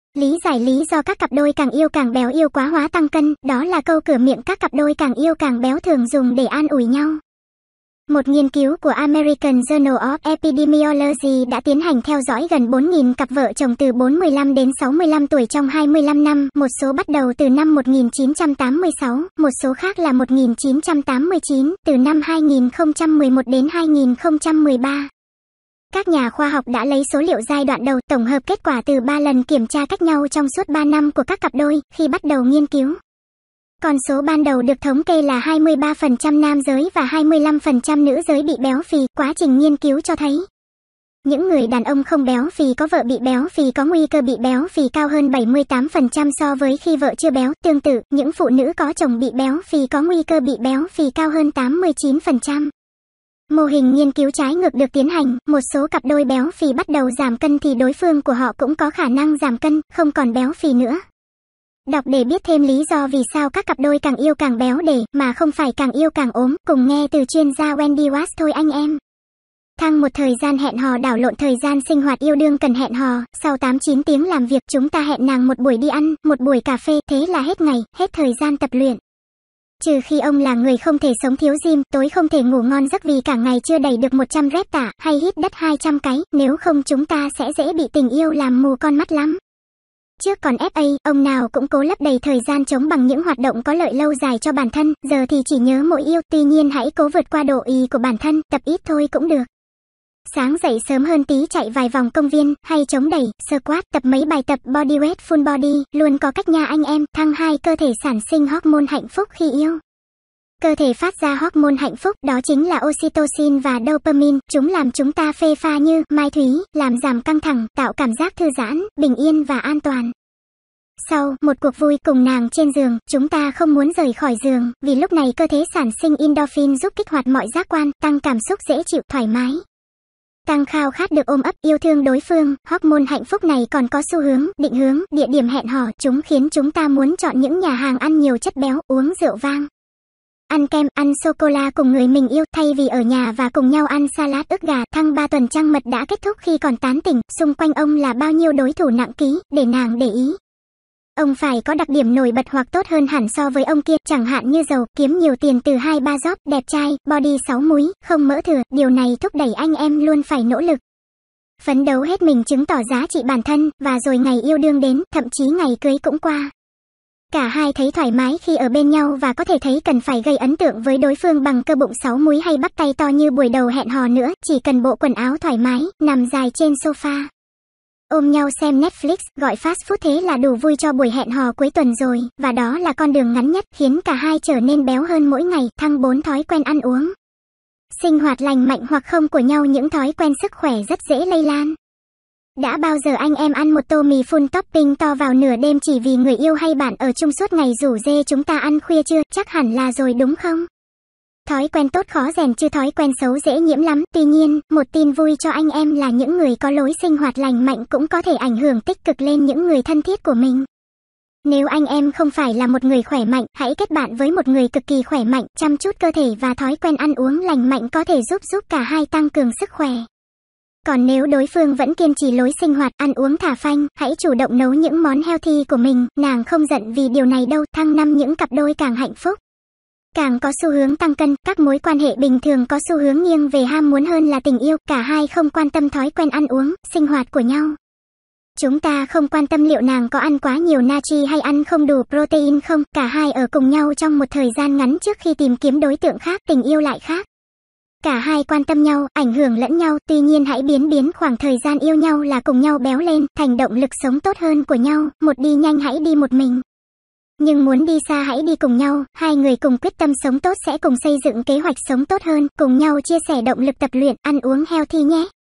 Lý giải lý do các cặp đôi càng yêu càng béo
Yêu quá hóa tăng cân, đó là câu cửa miệng các cặp đôi càng yêu càng béo thường dùng để an ủi nhau.

Một nghiên cứu của American Journal of Epidemiology đã tiến hành theo dõi gần 4.000 cặp vợ chồng từ 45–65 tuổi trong 25 năm (một số bắt đầu từ năm 1986, một số khác là 1989).

Từ năm 2011 đến 2013, các nhà khoa học đã lấy số liệu giai đoạn đầu, tổng hợp kết quả từ 3 lần kiểm tra cách nhau trong suốt ba năm của các cặp đôi.

Khi bắt đầu nghiên cứu, con số ban đầu được thống kê là 23% nam giới và 25% nữ giới bị béo phì.

Quá trình nghiên cứu cho thấy, những người đàn ông không béo phì có vợ bị béo phì có nguy cơ bị béo phì cao hơn 78% so với khi vợ chưa béo.

Tương tự, những phụ nữ có chồng bị béo phì có nguy cơ bị béo phì cao hơn 89%.

Mô hình nghiên cứu trái ngược được tiến hành, một số cặp đôi béo phì bắt đầu giảm cân thì đối phương của họ cũng có khả năng giảm cân, không còn béo phì nữa.

Đọc để biết thêm lý do vì sao các cặp đôi càng yêu càng béo để, mà không phải càng yêu càng ốm, cùng nghe từ chuyên gia Wendy Walsh thôi anh em.

#1 Thời gian hẹn hò đảo lộn thời gian sinh hoạt

Yêu đương cần hẹn hò, sau 8 9 tiếng làm việc, chúng ta hẹn nàng một buổi đi ăn, một buổi cà phê, thế là hết ngày, hết thời gian tập luyện.

Trừ khi ông là người không thể sống thiếu gym, tối không thể ngủ ngon giấc vì cả ngày chưa đẩy được 100 reps tạ, hay hít đất 200 cái. Nếu không chúng ta sẽ dễ bị tình yêu làm mù con mắt lắm.

Trước còn FA, ông nào cũng cố lấp đầy thời gian trống bằng những hoạt động có lợi lâu dài cho bản thân. Giờ thì chỉ nhớ mỗi yêu.

Tuy nhiên hãy cố vượt qua độ ì của bản thân, tập ít thôi cũng được, sáng dậy sớm hơn tí chạy vài vòng công viên, hay chống đẩy, squat, tập mấy bài tập bodyweight full-body, luôn có cách nha anh em.

#2 Cơ thể sản sinh hormone hạnh phúc

Khi yêu, cơ thể phát ra hormone hạnh phúc, đó chính là oxytocin và dopamine. Chúng làm chúng ta phê pha như “mai thúy”, làm giảm căng thẳng, tạo cảm giác thư giãn, bình yên và an toàn.

Sau “một cuộc vui” cùng nàng trên giường, chúng ta không muốn rời khỏi giường, vì lúc này cơ thế sản sinh endorphin giúp kích hoạt mọi giác quan, tăng cảm xúc dễ chịu, thoải mái, tăng khao khát được ôm ấp, yêu thương đối phương.

Hormone hạnh phúc này còn có xu hướng “định hướng” địa điểm hẹn hò. Chúng khiến chúng ta muốn chọn những nhà hàng ăn nhiều chất béo, uống rượu vang, ăn kem, ăn socola cùng người mình yêu, thay vì ở nhà và cùng nhau ăn salad ức gà.

#3 Tuần trăng mật đã kết thúc

Khi còn tán tỉnh, xung quanh ông là bao nhiêu đối thủ nặng ký? Để nàng để ý, ông phải có đặc điểm nổi bật hoặc tốt hơn hẳn so với ông kia.

Chẳng hạn như giàu, kiếm nhiều tiền từ 2 3 job, đẹp trai, body 6 múi, không mỡ thừa,... Điều này thúc đẩy anh em luôn phải nỗ lực, phấn đấu hết mình chứng tỏ giá trị bản thân.

Và rồi ngày yêu đương đến, thậm chí ngày cưới cũng qua, cả hai thấy thoải mái khi ở bên nhau và có thể thấy cần phải gây ấn tượng với đối phương bằng cơ bụng 6 múi hay bắp tay to như buổi đầu hẹn hò nữa.

Chỉ cần bộ quần áo thoải mái, nằm dài trên sofa, ôm nhau xem Netflix, gọi fast food thế là đủ vui cho buổi hẹn hò cuối tuần rồi.

Và đó là con đường ngắn nhất, khiến cả hai trở nên béo hơn mỗi ngày.

#4 Thói quen ăn uống, sinh hoạt lành mạnh hoặc không của nhau

Những thói quen sức khỏe rất dễ lây lan. Đã bao giờ anh em ăn một tô mì full topping to vào nửa đêm chỉ vì người yêu hay bạn ở chung suốt ngày rủ rê chúng ta ăn khuya chưa? Chắc hẳn là rồi đúng không?

Thói quen tốt khó rèn chứ thói quen xấu dễ nhiễm lắm.

Tuy nhiên, một tin vui cho anh em là những người có lối sinh hoạt lành mạnh cũng có thể ảnh hưởng tích cực lên những người thân thiết của mình.

Nếu anh em không phải là một người khỏe mạnh, hãy kết bạn với một người cực kỳ khỏe mạnh, chăm chút cơ thể và thói quen ăn uống lành mạnh có thể giúp giúp cả hai tăng cường sức khỏe.

Còn nếu đối phương vẫn kiên trì lối sinh hoạt, ăn uống thả phanh, hãy chủ động nấu những món healthy của mình. Nàng không giận vì điều này đâu.

#5 Những cặp đôi càng hạnh phúc, càng có xu hướng tăng cân?

Các mối quan hệ bình thường có xu hướng nghiêng về ham muốn hơn là tình yêu. Cả hai không quan tâm thói quen ăn uống, sinh hoạt của nhau.

Chúng ta không quan tâm liệu nàng có ăn quá nhiều natri hay ăn không đủ protein không. Cả hai ở cùng nhau trong một thời gian ngắn trước khi tìm kiếm đối tượng khác.

Tình yêu lại khác, cả hai quan tâm nhau, ảnh hưởng lẫn nhau. Tuy nhiên hãy biến biến khoảng thời gian yêu nhau là cùng nhau béo lên, thành động lực sống tốt hơn của nhau.

Một đi nhanh hãy đi một mình, nhưng muốn đi xa hãy đi cùng nhau. 2 người cùng quyết tâm sống tốt sẽ cùng xây dựng kế hoạch sống tốt hơn. Cùng nhau chia sẻ động lực tập luyện, ăn uống healthy nhé!